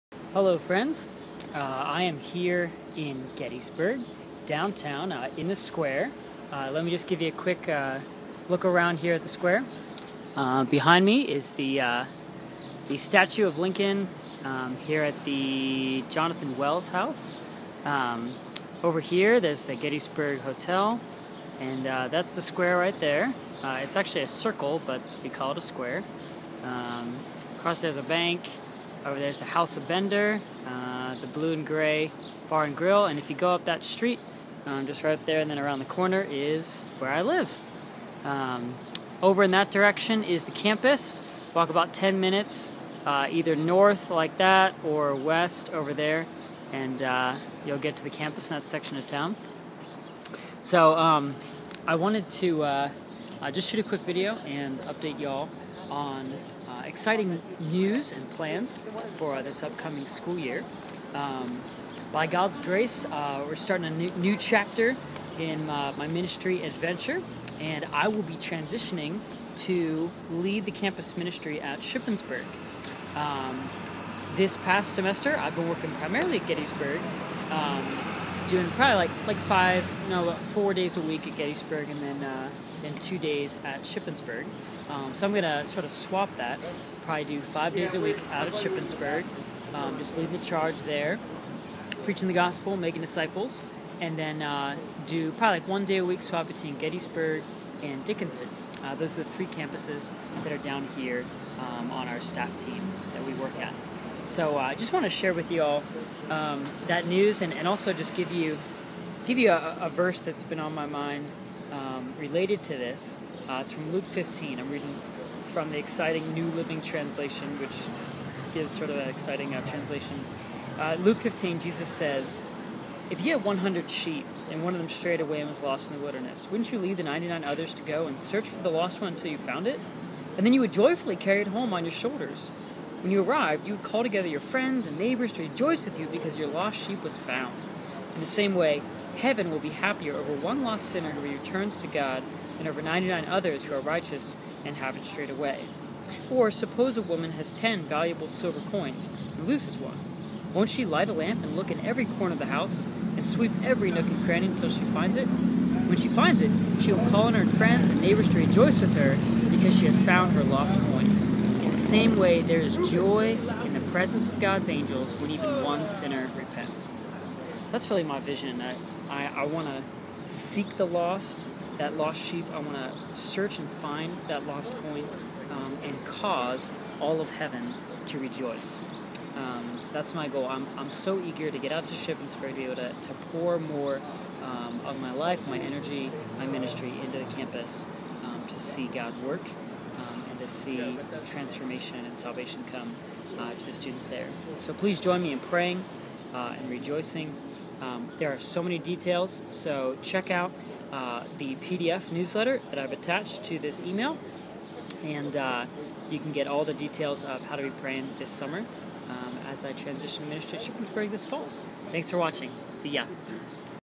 Anchors Aweigh: Transitioning to Shippensburg!
Exciting news! By God's grace I will be transitioning to lead the charge at Shippensburg for this next school year. I'm eager to follow Luke 15 and seek the lost so that I might cause heaven to rejoice!

***Correction: that's the David Wills house, not Jonathan Wells :P